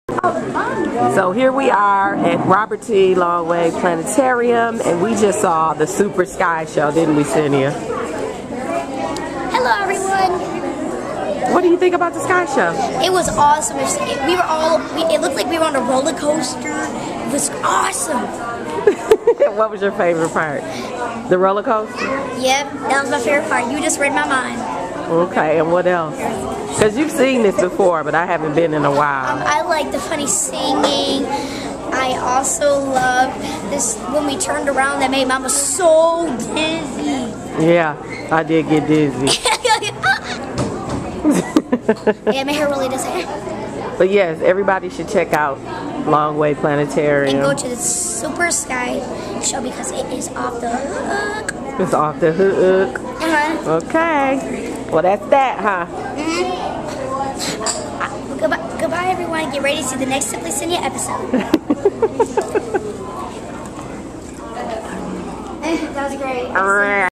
Senia Loves Longway Planetarium
Senia enjoys the Super Sky Show at Longway Planetarium.